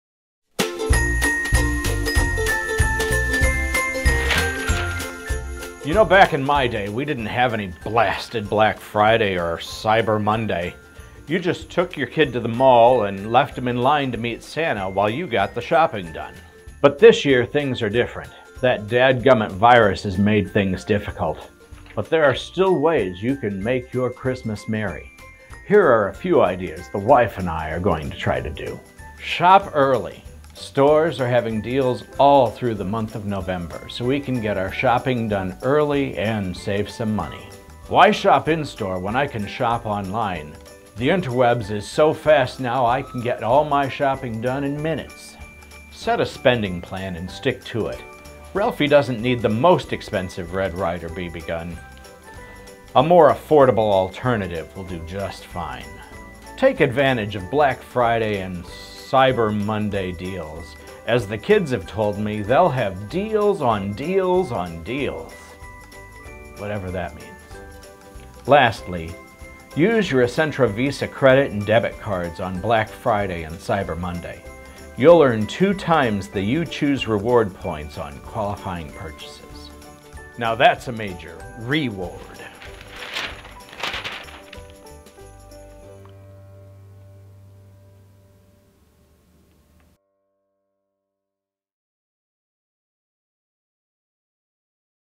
Hol ee day Tips Black Friday and Cyber Monday
Old Man Parker has a few tips to help you through Black Friday and Cyber Monday. Now that's a major RE-ward.